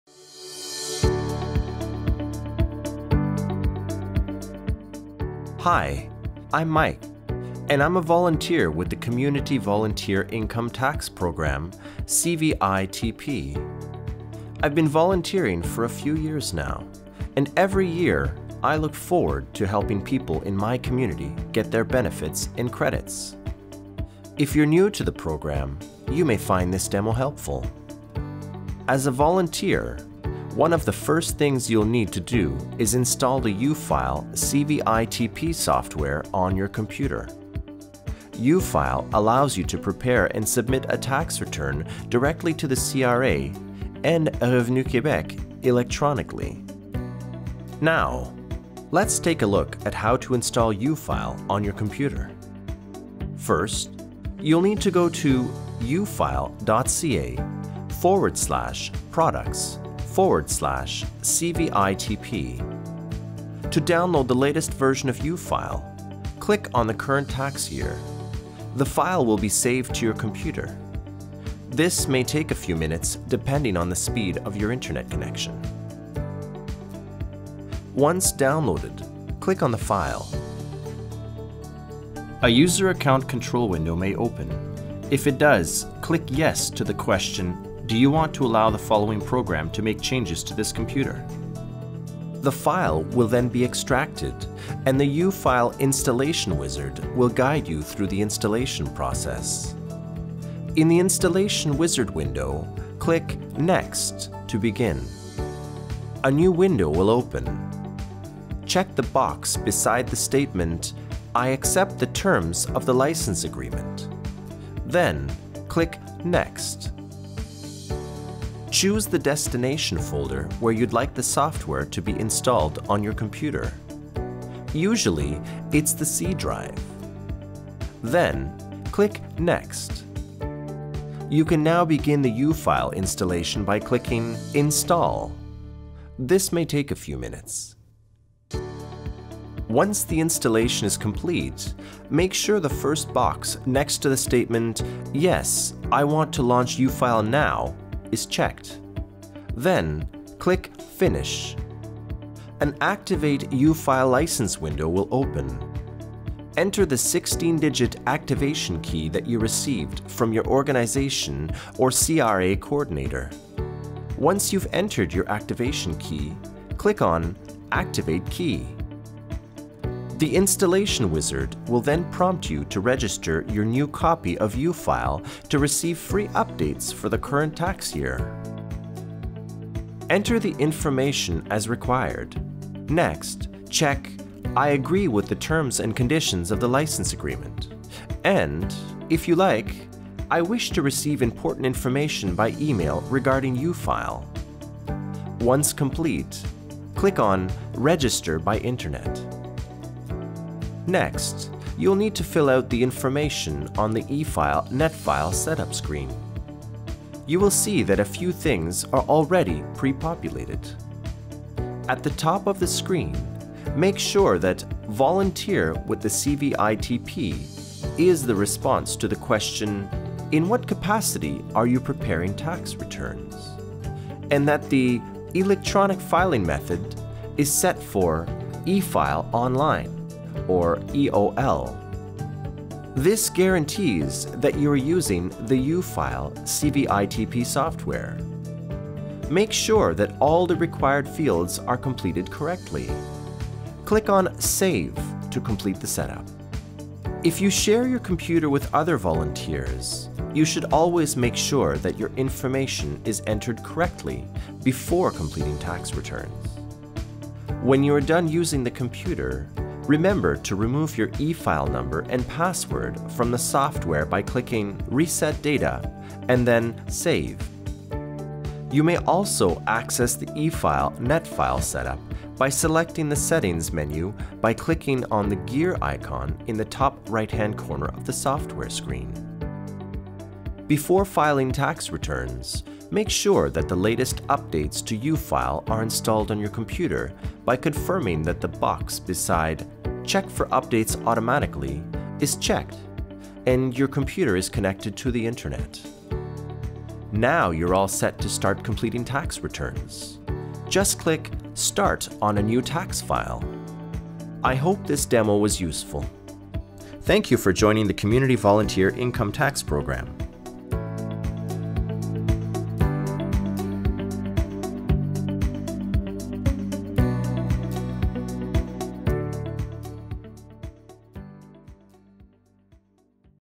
How to Install the UFile CVITP Software
Hi, I’m Mike, and I’m a volunteer with the Community Volunteer Income Tax Program (CVITP). I’ve been volunteering for a few years now and, every year, I look forward to helping people in my community get their benefits and credits.

If you’re new to the program, you may find this demo helpful.

As a CVITP volunteer, one of the first things you’ll need to do is install the UFile CVITP software on your computer.